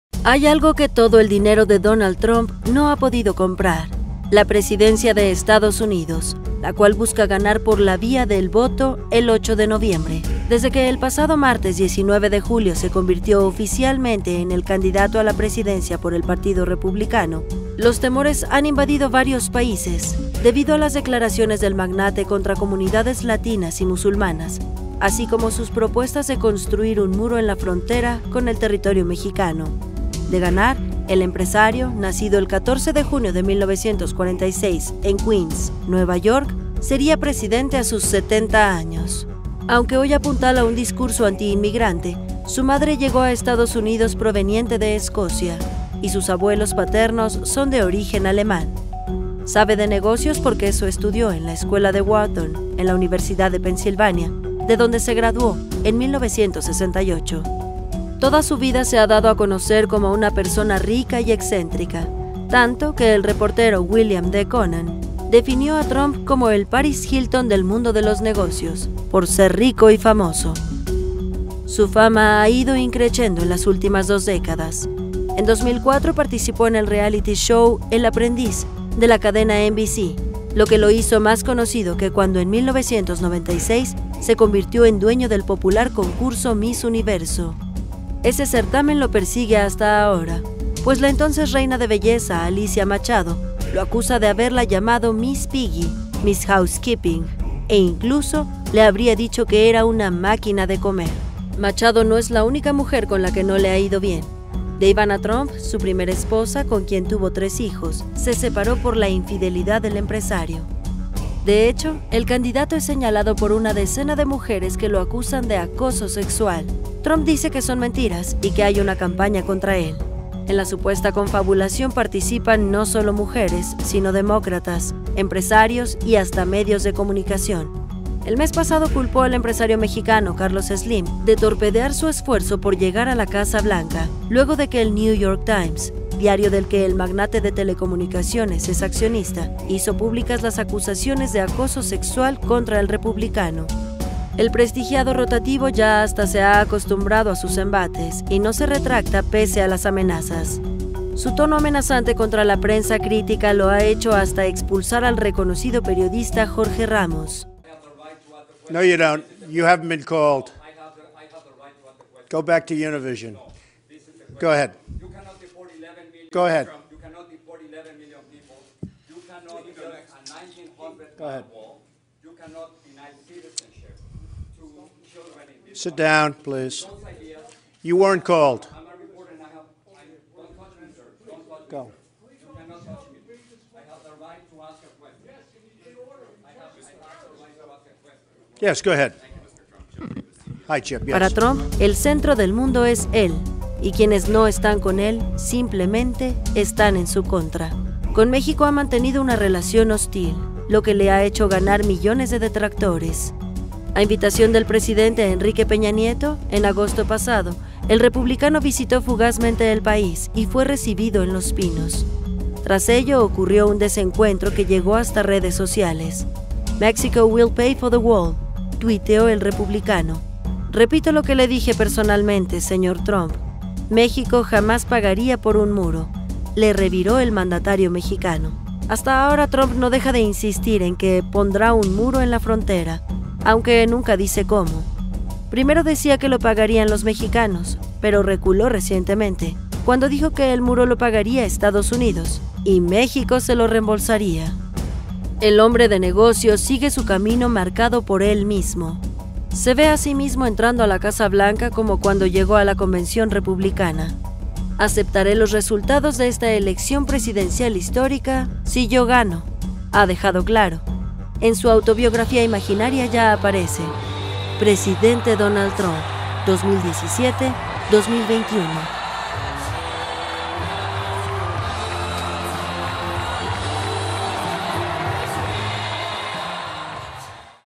Donald Trump: el “Paris Hilton” de la política, a un paso de la Casa Blanca
“Aceptaré los resultados de esta elección presidencial histórica... si yo gano”, ya ha advertido el candidato republicano. Ir a la nota